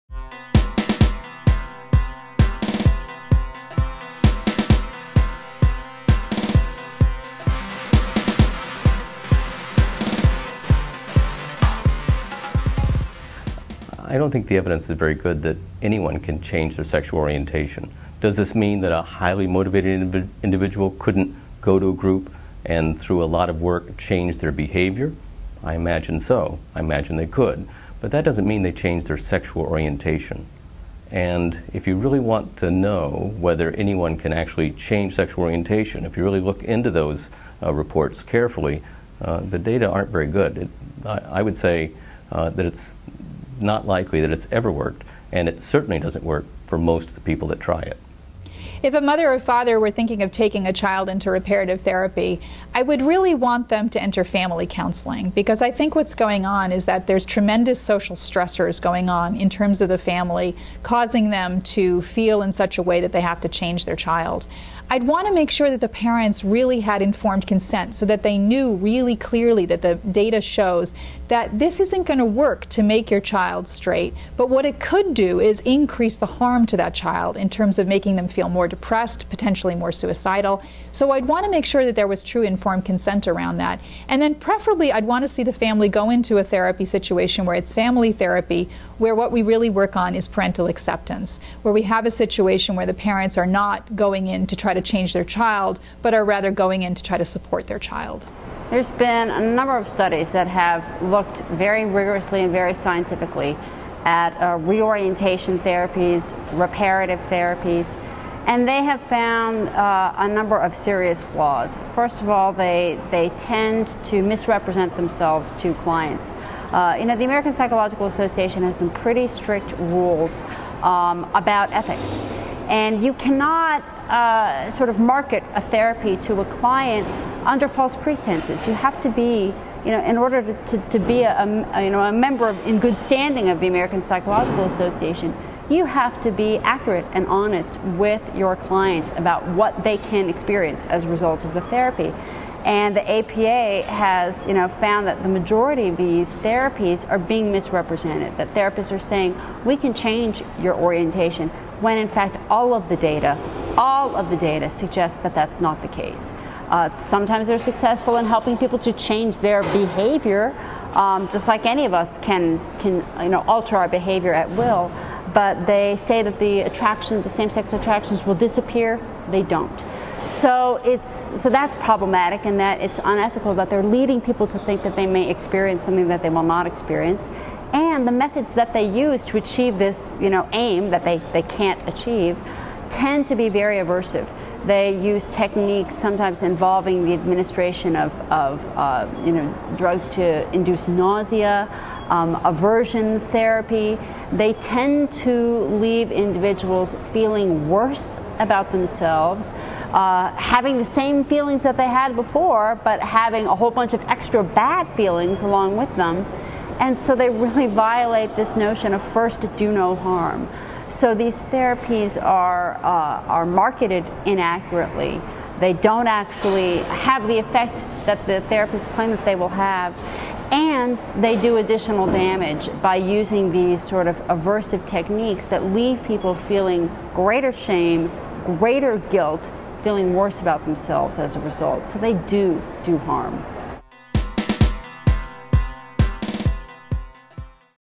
Science: 'Ex-Gay' Programs Do Not Work and Can be Harmful
Leading scientists who study sexual orientation say that "ex-gay" programs are futile and can cause real harm to those who try to change from gay to straight